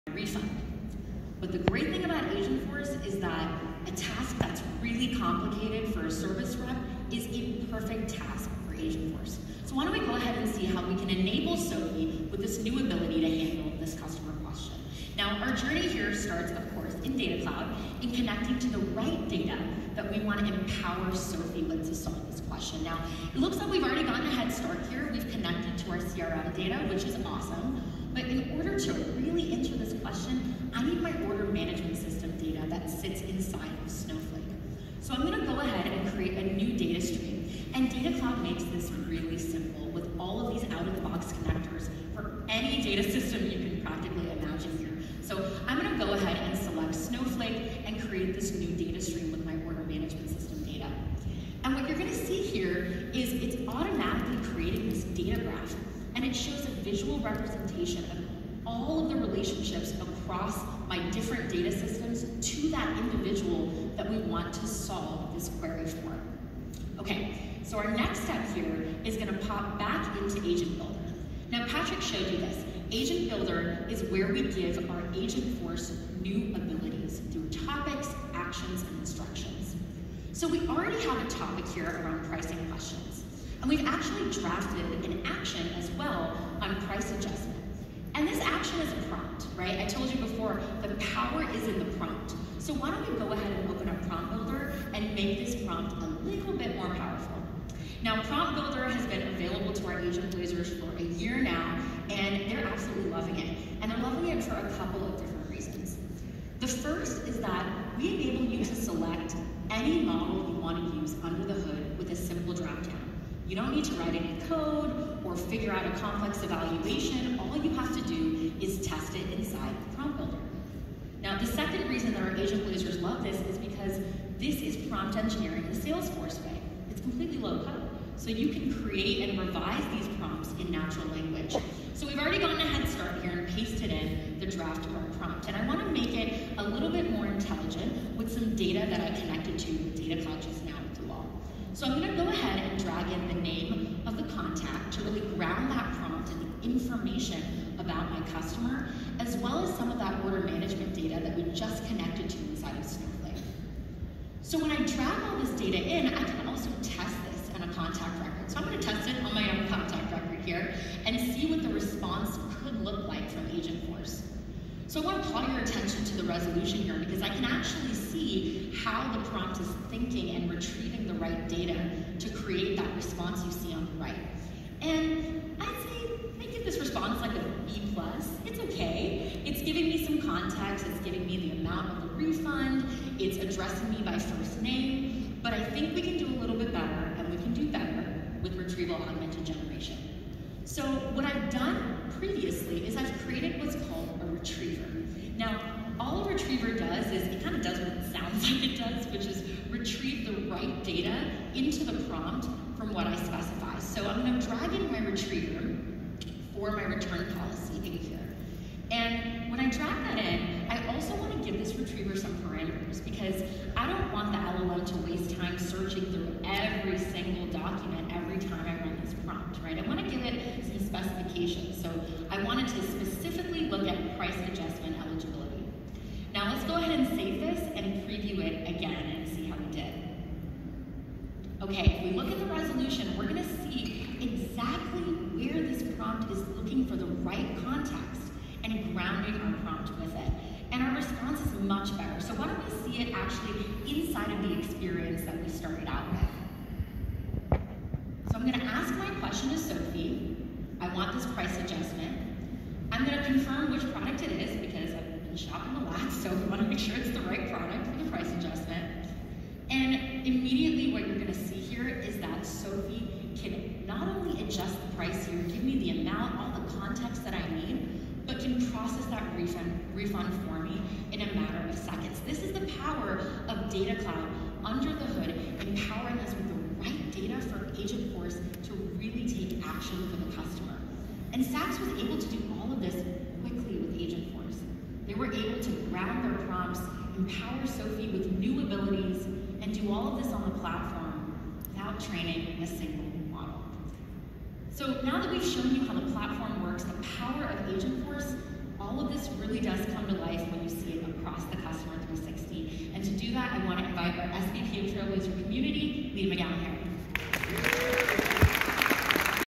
Sanjna Parulekar VP of Product Marketing @salesforce demos Sophie price matching item w/Agentforce
Sanjna Parulekar VP of Product Marketing ‎Salesforce demos Saks Customer Service Agent Sophie price matching and refunding the difference on an item for a customer using Agentforce.